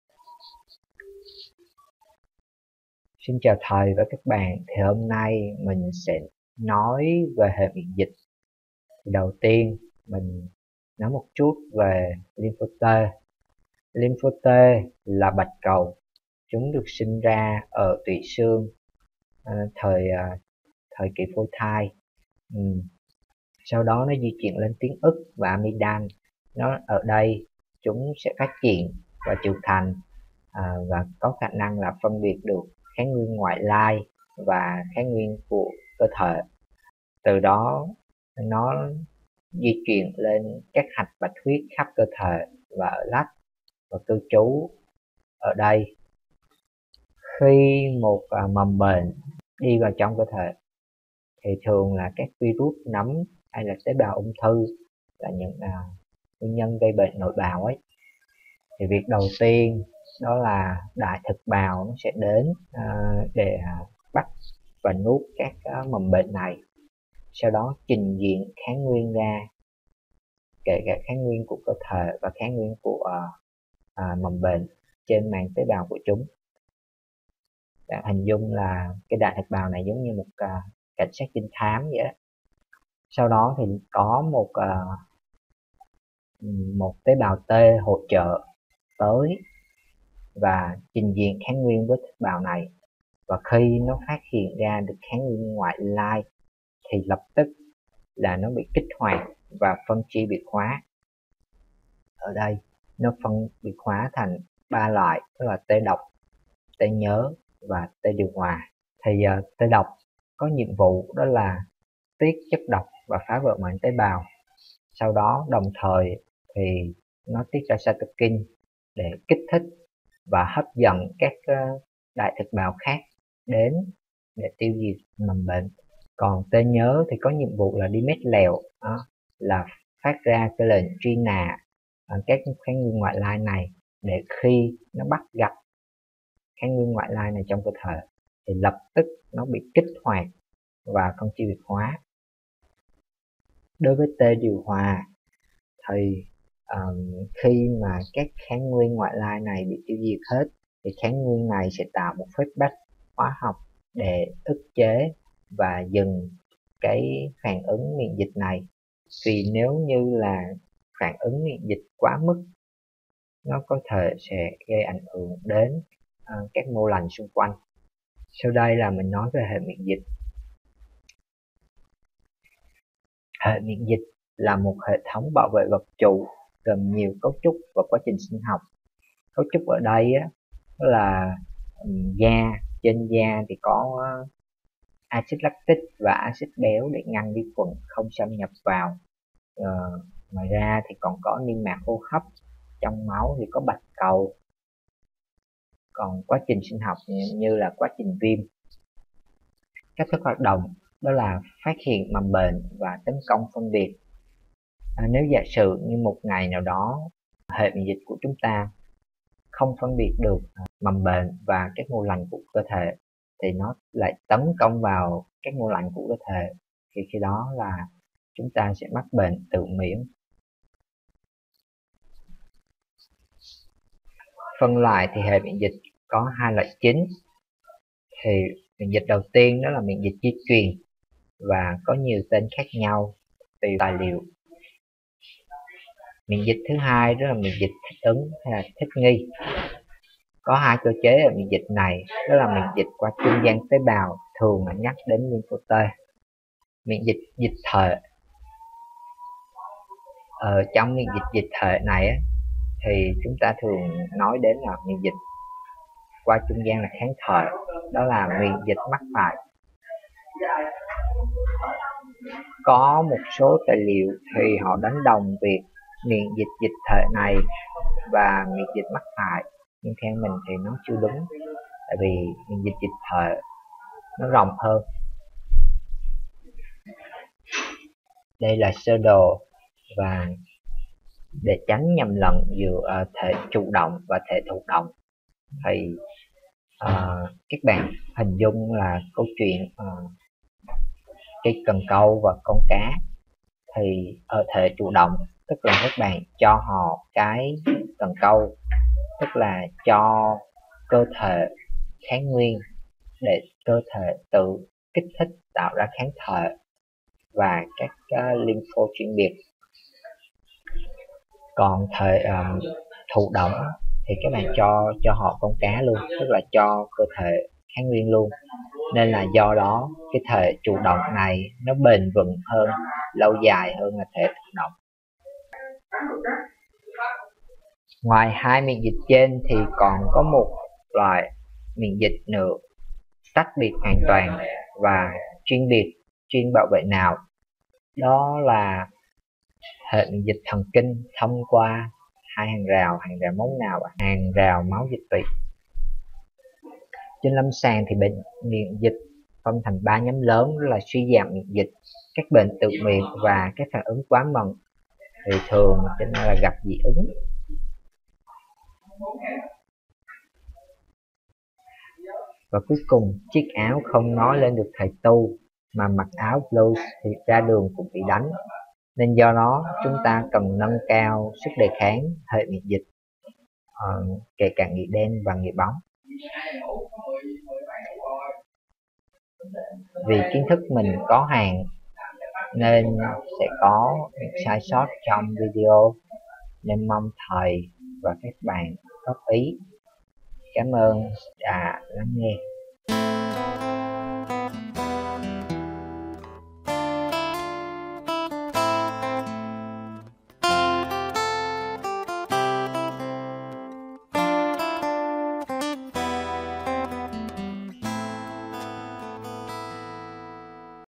5-4-2019, Minh Trí, y43, trình bày tb T và hệ miễn dịch.
Minh Trí, Y43, lớp Phúc Đinh